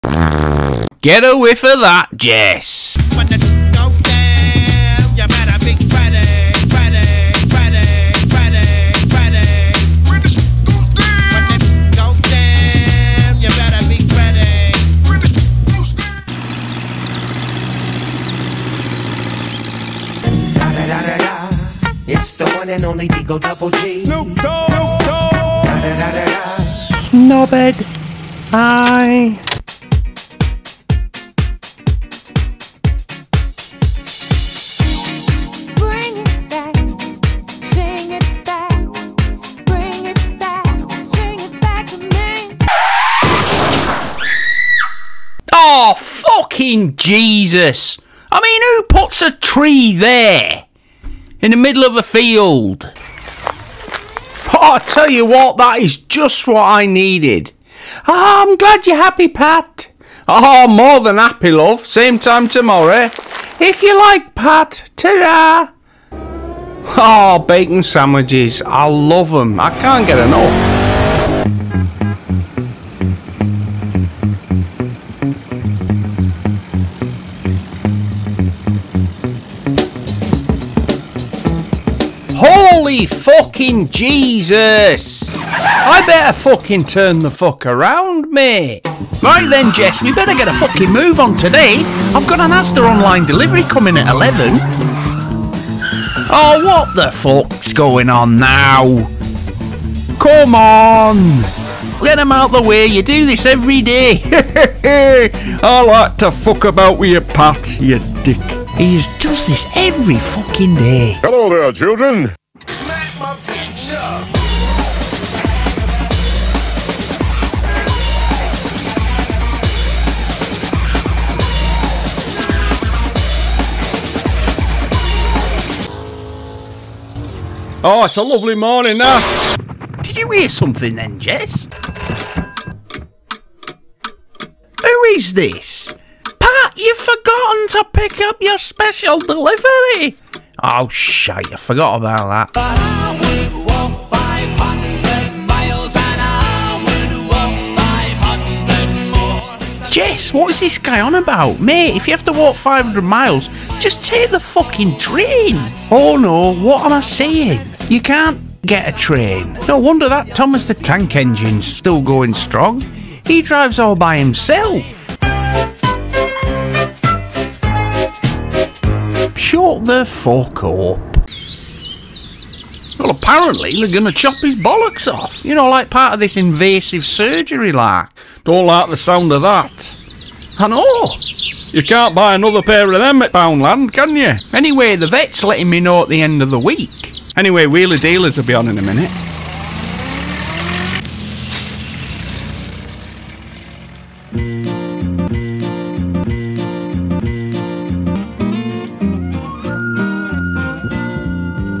Pissed Off Postman Pat FUNKY DELIVERY OMNIBUS
Follow this musical Pissed off Postman Pat as he drives along delivering all the shit in his van.

The real life Pissed Off Postman Pat for sure!

Though not to neglect other delivery companies, this video pays homage to Parcel Force, DHL, EVRI, Yodel, UPS, Amazon and everyone else involved in this most hectic time of the year.

If you know someone who delivers parcels or works (sometimes) for Royal Mail, then send them a link to this video of Jamaica Pissed Off Postman Pat. Enjoy!